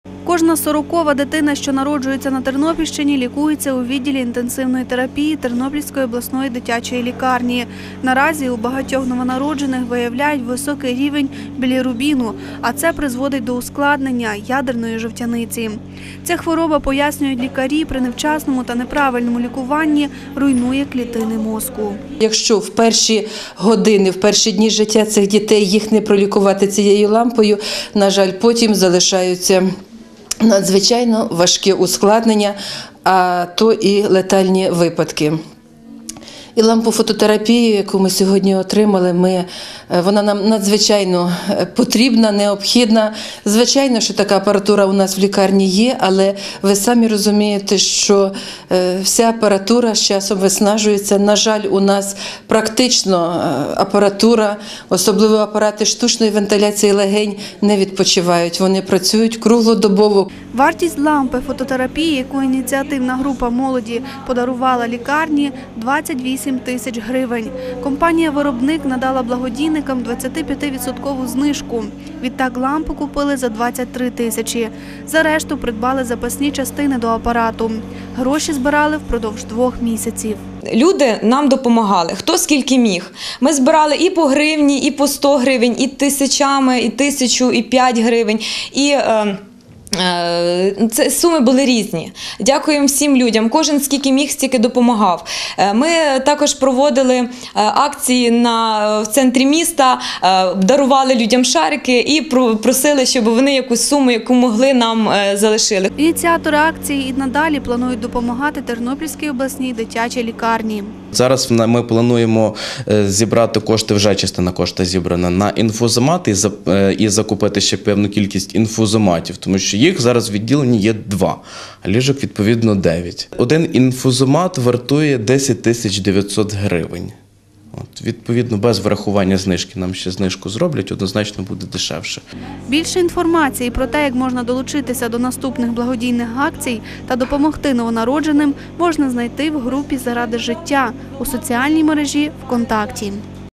Дитяча обласна лікарня отримала фотоламмпу
Лампу фототерапії, яка вкрай необхідна для дітей, що народилися з жовтяницею, ініціативна група молодих тернополян подарувала відділу інтенсивної терапії новонароджених Тернопільської обласної дитячої клінічної лікарні. Вартість такого апарату - більше 23 тисяч гривень. Збирали гроші  під гаслом проекту "Здорові діти - щаслива Україна", що приєднався до благодійного проекту "Заради життя".